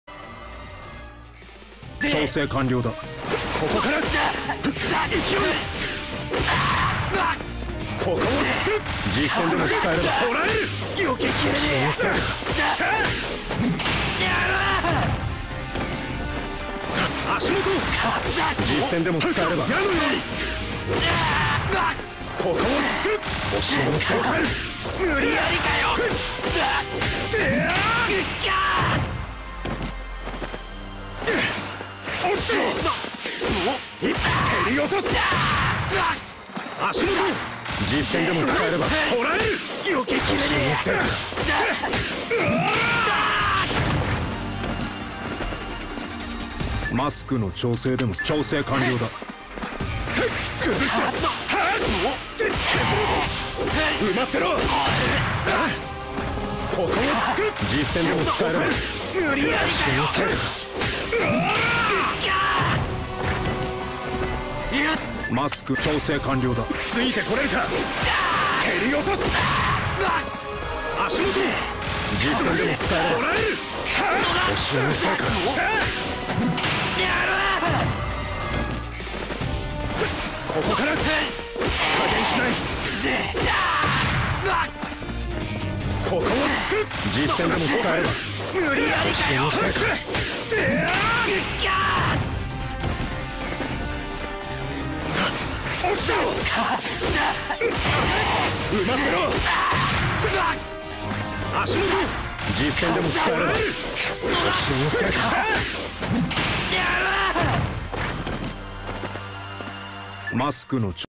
Hitoshi Shinso Bread and Butter combos
I was for real thinking of never making this video, but I guess at some point in the future I would have to do so screw it, right?

Ground meterless BnB:
AA, TA, TQ1, UB, AA, TQ2(Pull), UB, AA, TQ2 TQ2

Air Meterless BnB:
J.AAAA, TQ1, Land, UB, AA, TQ2(Pull), UB, AA, TQ2 TQ2

Air Meterless V2 (Height Depended)
J.AAA, TA(Two hits), TQ1, UB, A, TQ2(Pull), UB, A, TQ2 TQ2

Starting with J.TQ2 TQ2
J.TQ2 TQ2, DC, TQ1, UB, AA, TQ2(pull), UB, AA, TQ2 TQ2

DC Ground BnB:
AAA, Q1, DC, TQ1, UB, A, TQ2 TQ2